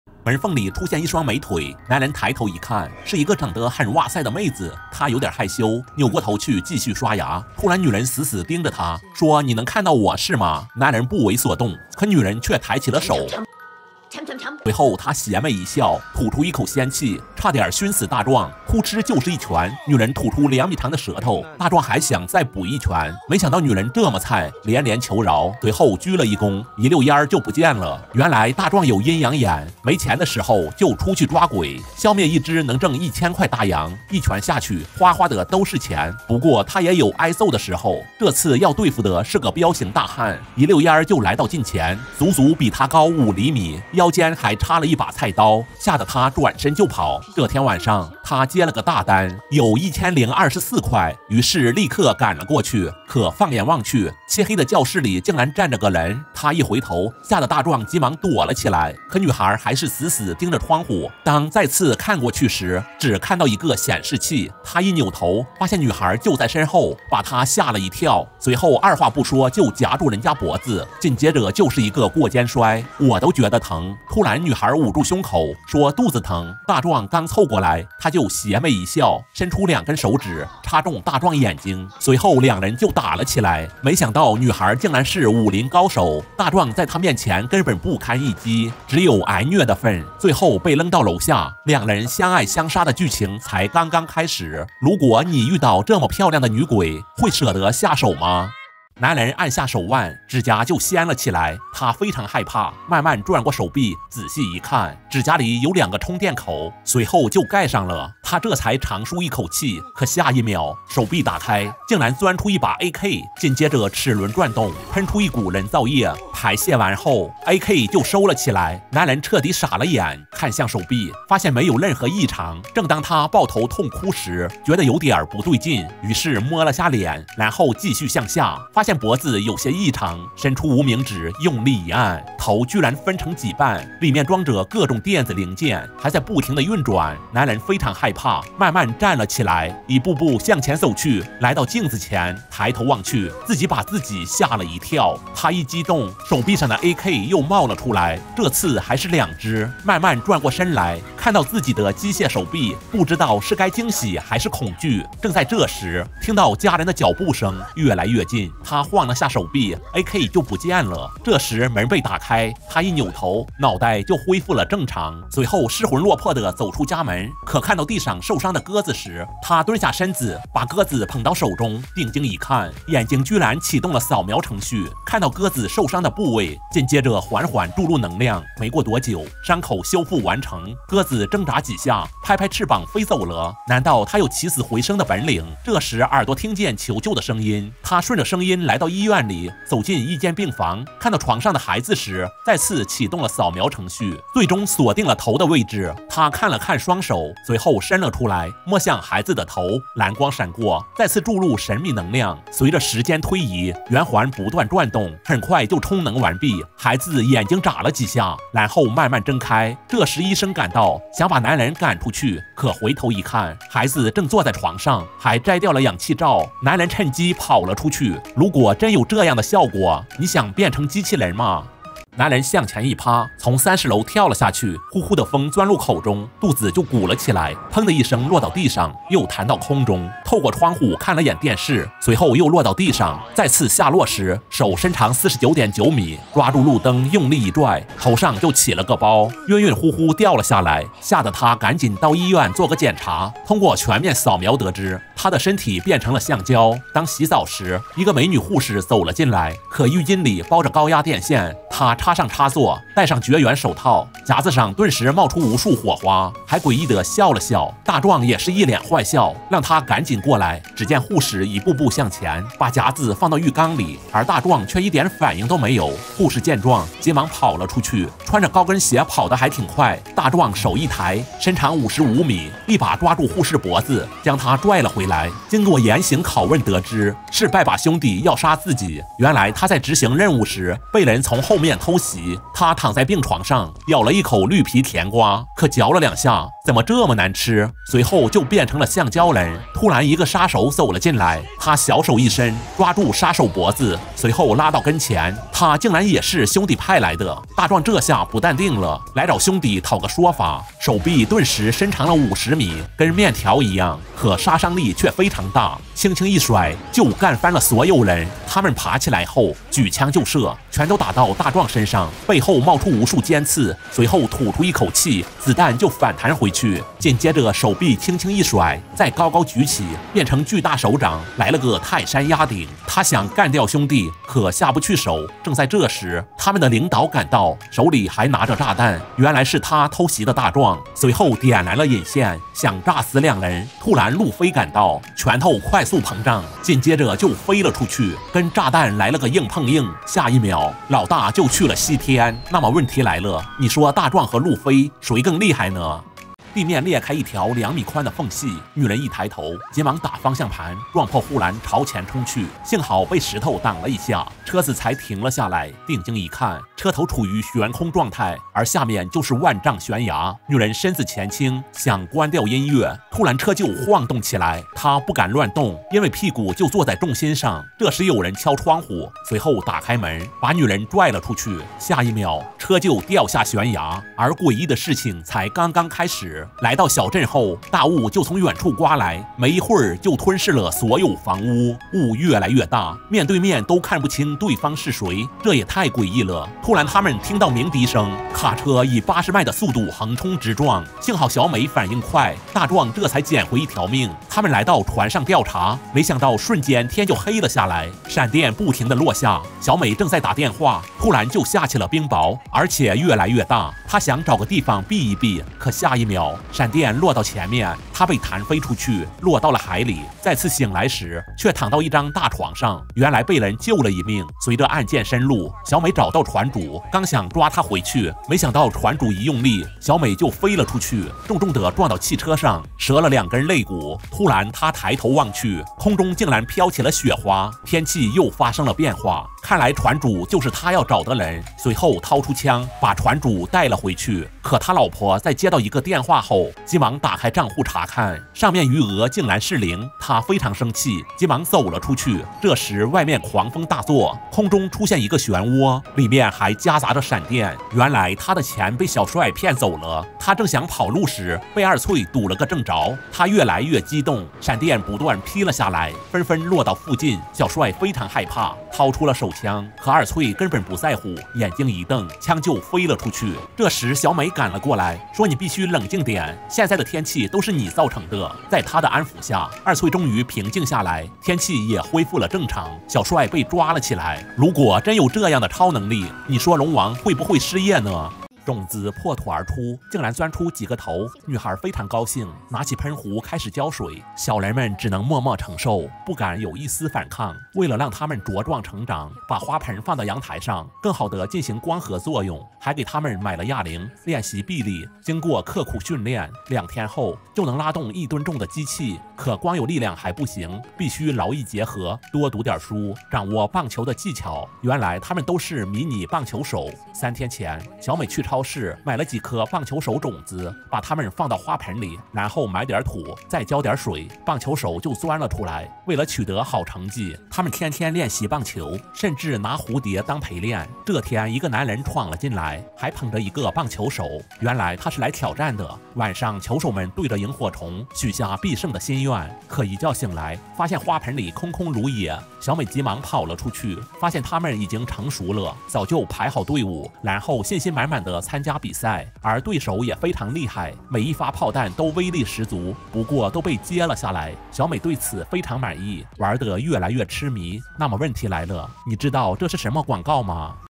女鬼晚上出來嚇人，卻被胖揍一頓，科幻，奇幻，電影，解說，獵奇，喜劇，搞笑，超能力，短片，short films
女鬼晚上出來嚇人，卻被胖揍一頓，科幻，奇幻，電影，解說，獵奇，喜劇，搞笑，超能力，短片，short film 。